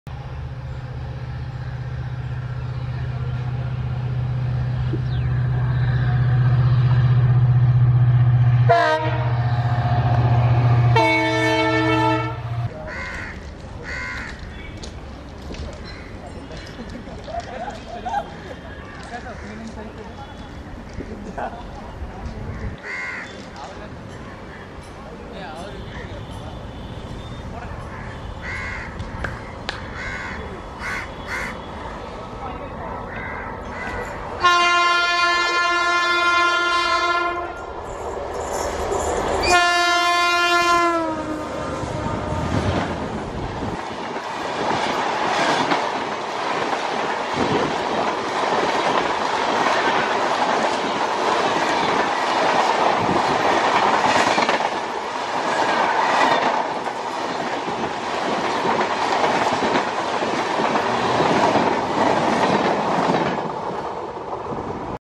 Thirukural Express | Delhi to Kanniyakumari train with ED WAP-7
12642/Thirukkural SF Express departs from Hazrat Nizamuddin at 05:20  and covers 2919  KM within 47h 20m hrs with average speed of 62 kmph

Location :st thomas mount

camera used Cannon sx 70HS